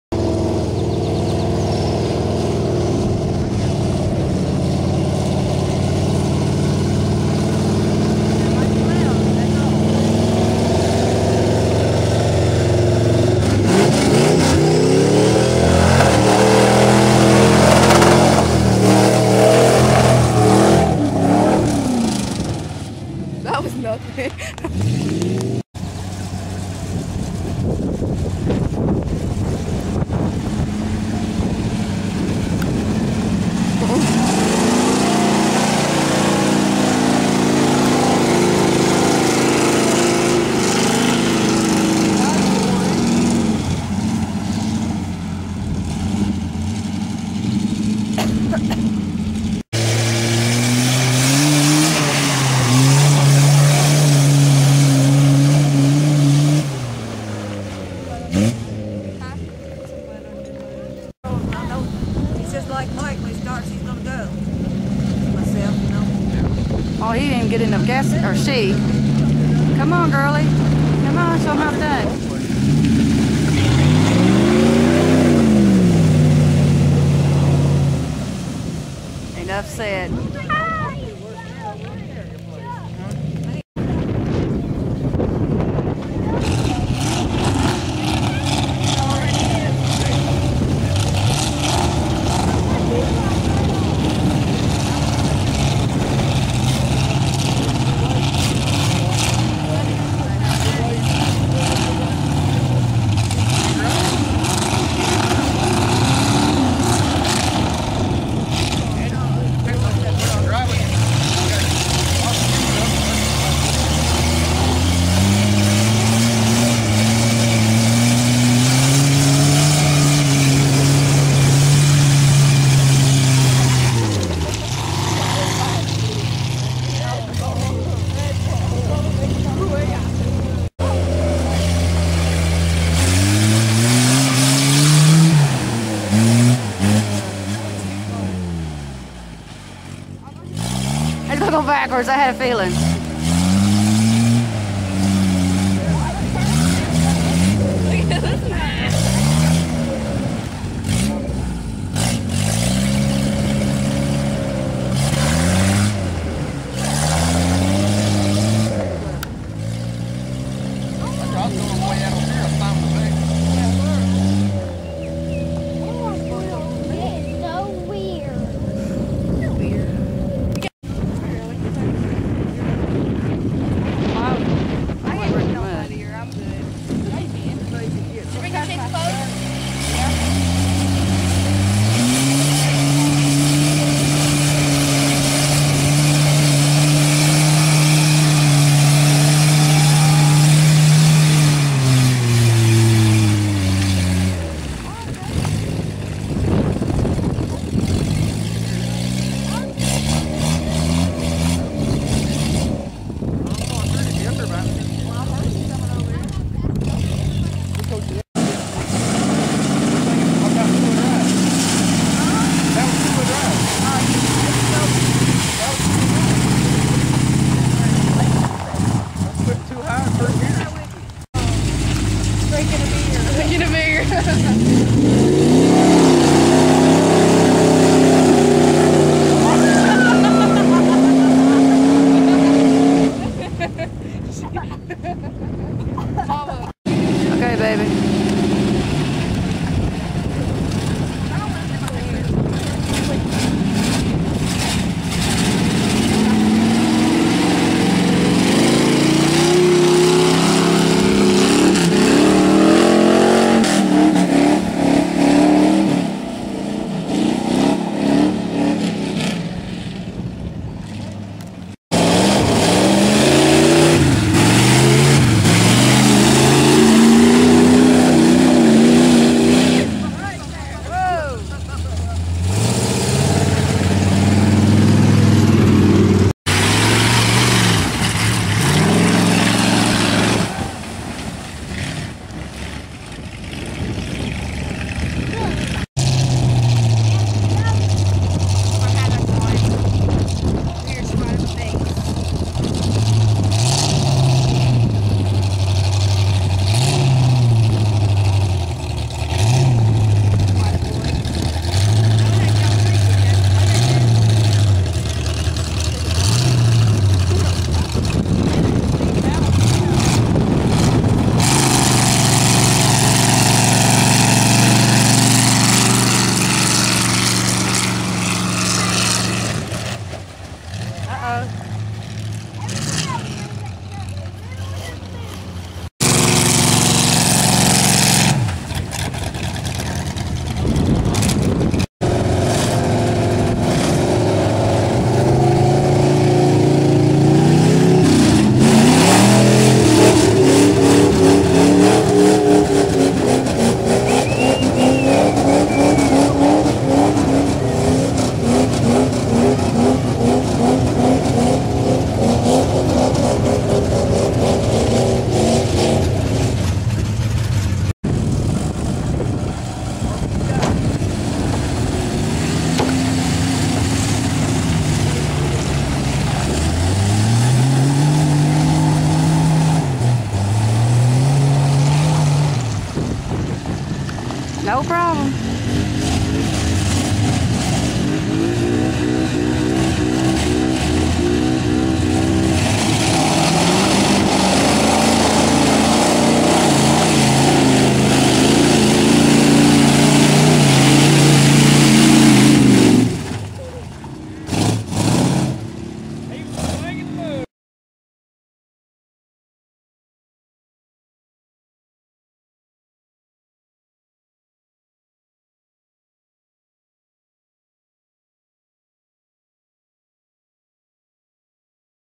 Flash Fest 2008 mud running
Flash Fest 08 2wd Mudhole It got deeper